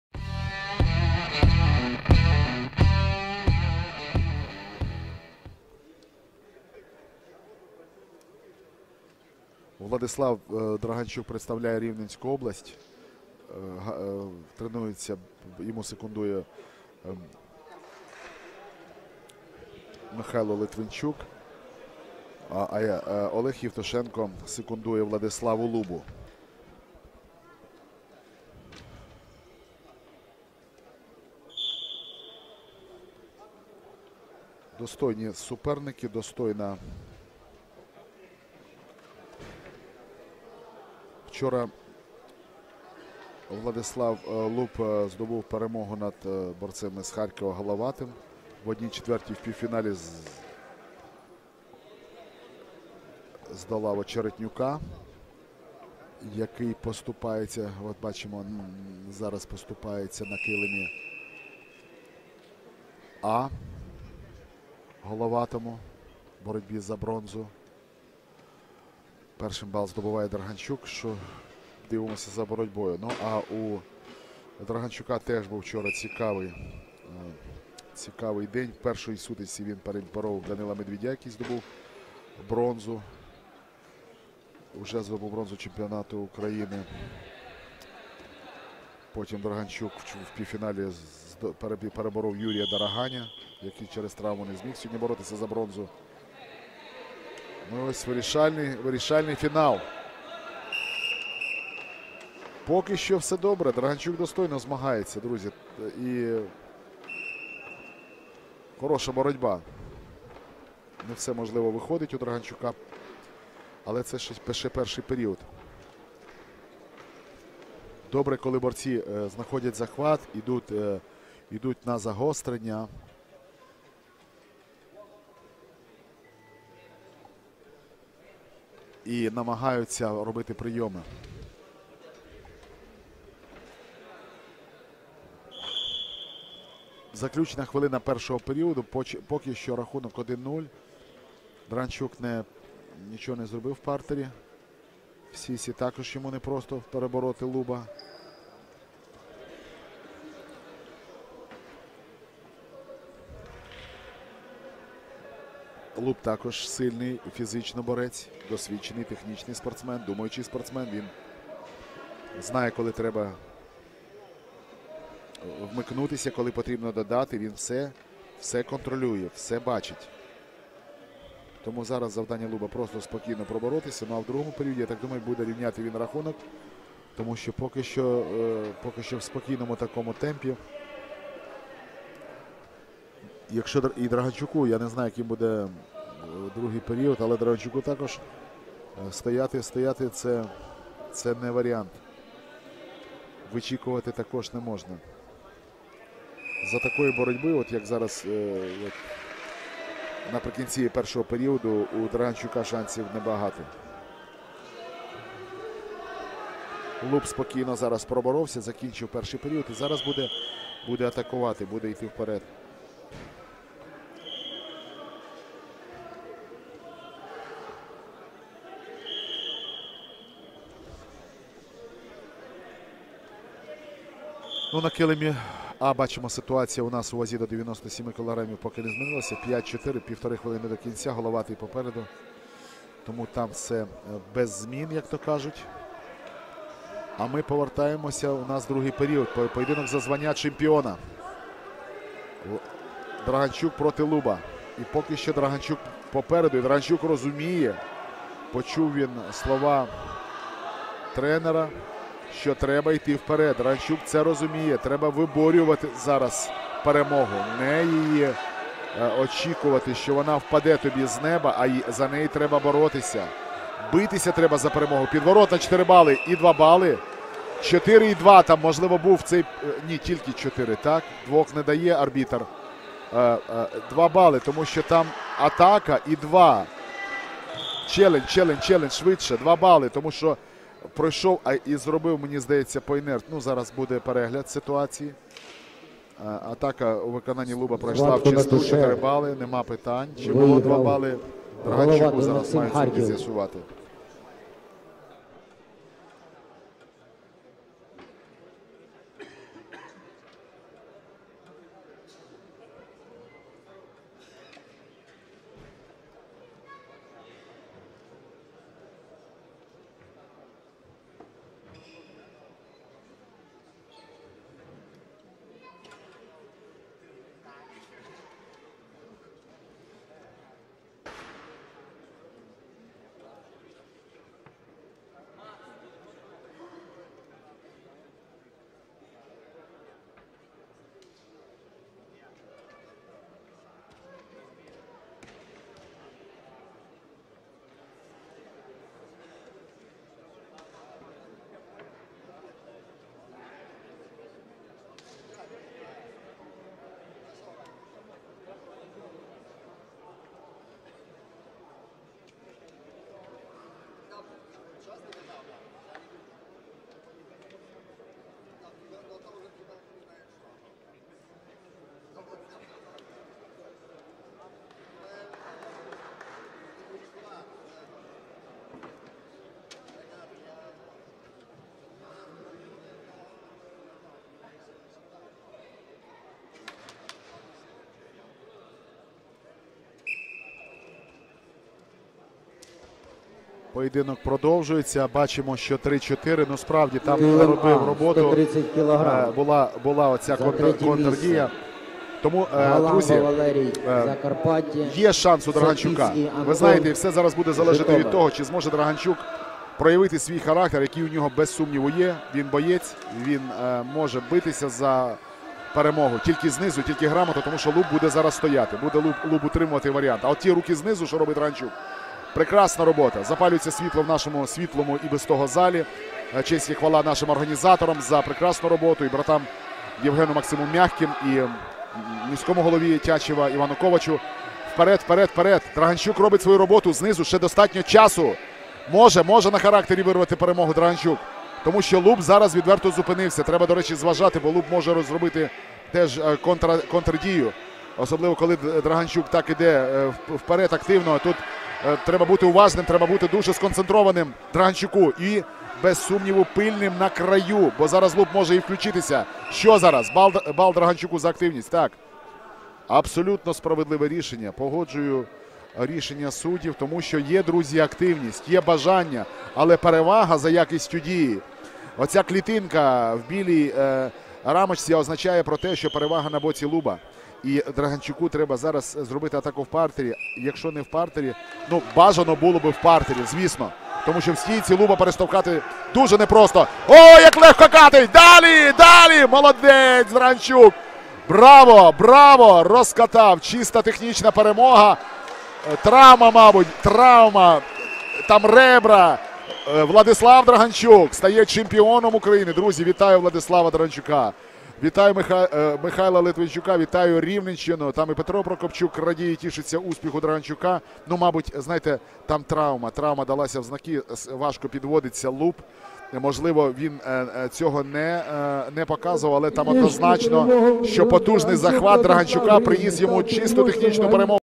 Драганчук - Луб (97) / Фінал Чемпіонату України U-20 / Тячів 2022
7-8 грудня 2022 р. в Тячеві відбувся Чемпіонат України з греко-римської боротьби серед борців до 20-ти років. У змаганнях взяли участь більше 130 спортсменів з усієї України.